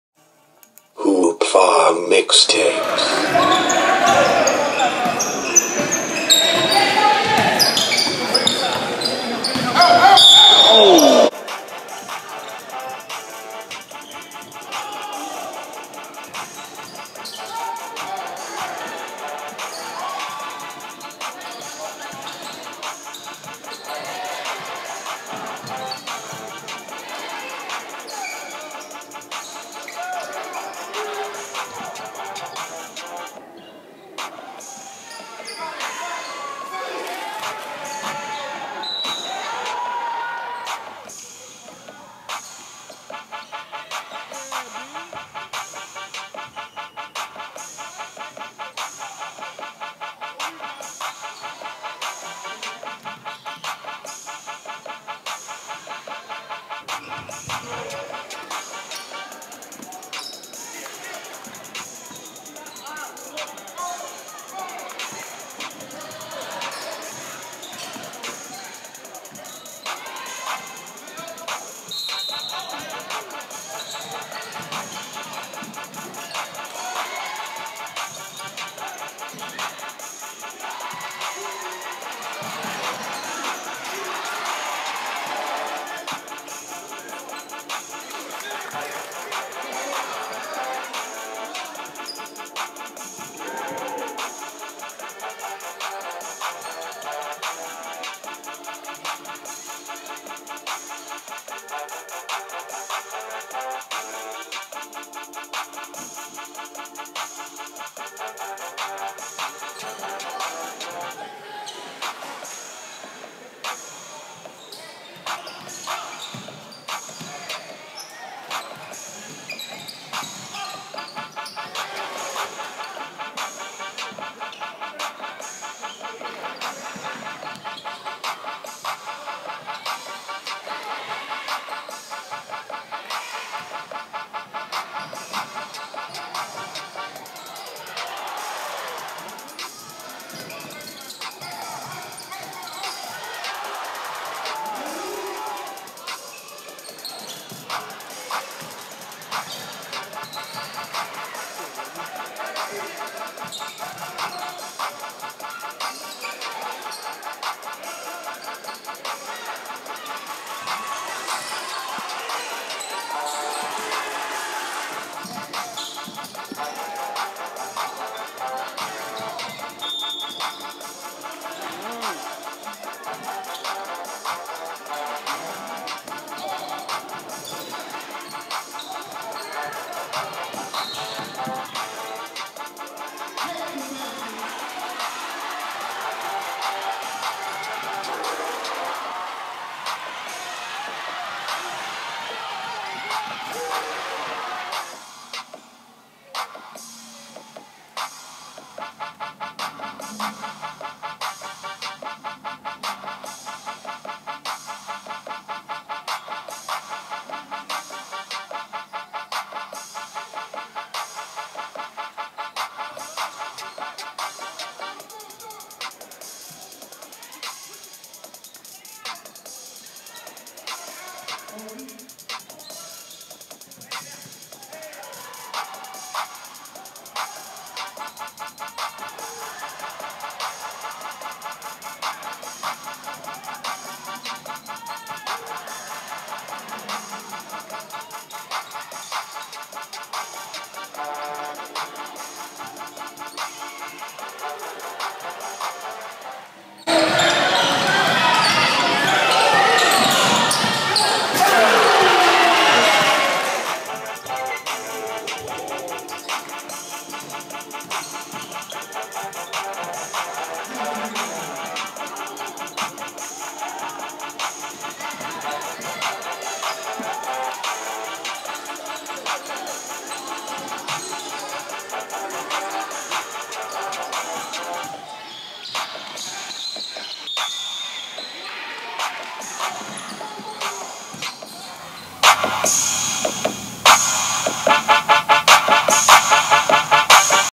2016 VA vs NC Unsigned Senior Showcase!!!
The 2016 VA vs NC unsigned senior showcase was held at Northeastern HS in Elizabeth City, NC. The game featured some of the top talent from the eastern part of NC ( Tyriq McClain, Ben Cox, Jahziah Dyer and Marlik Brooks) taking on the some of the top talent from right above the state border (Robert Smith, Josh Covington and Shawn Evans Jr)! Watch as both teams battle for bragging rights and to see who is the top state.